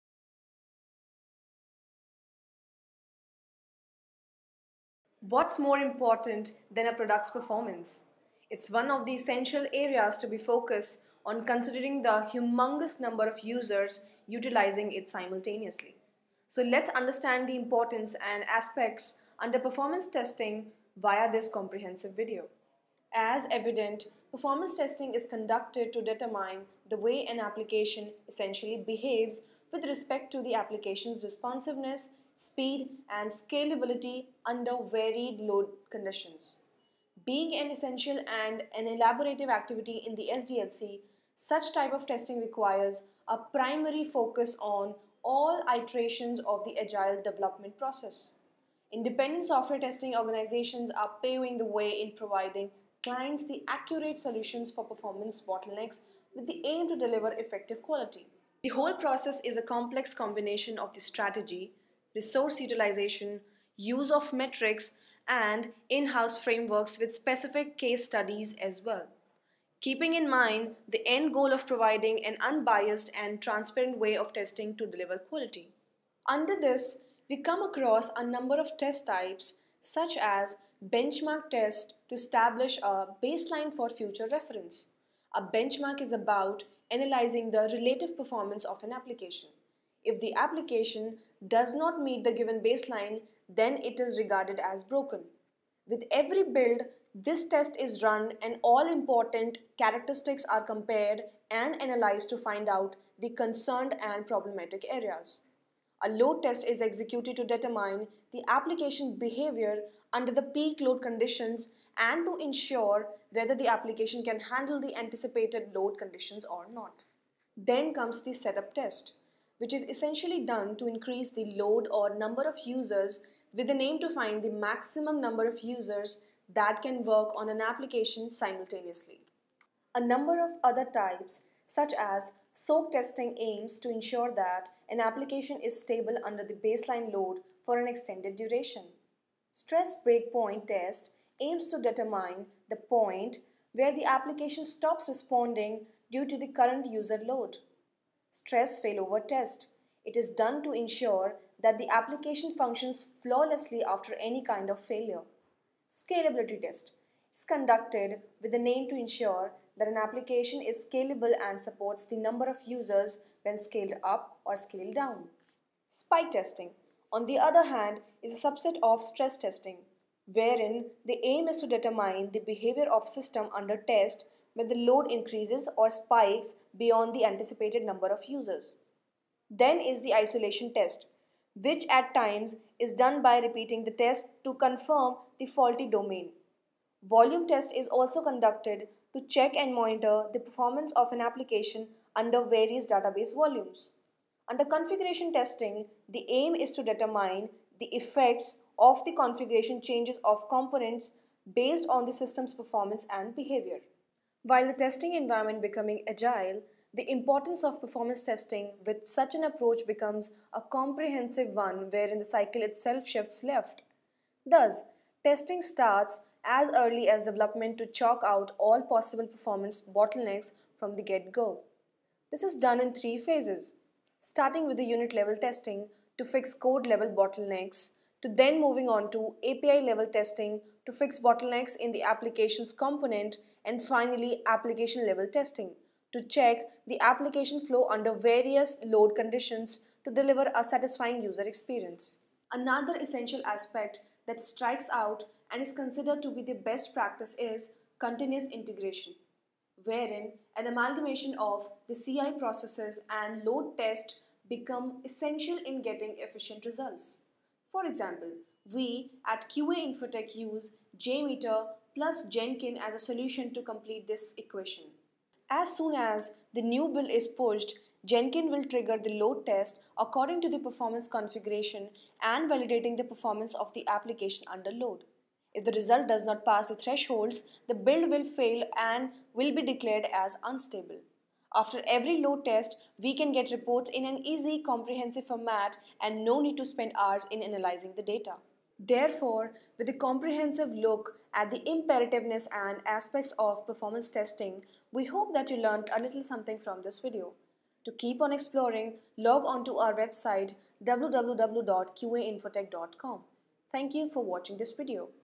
What is Performance Testing?
How does one ensure a satisfying user experience? The crucial step begins by ensuring flawless performance of one's application or product. This would not just ensure effective quality but would help in retaining customers for the longer run. With this video, our Thought Leaders will throw some light on software performance testing, performance testing, app performance testing, web performance testing, website load testing and performance tuning to help make your application ready to face a huge user traffic.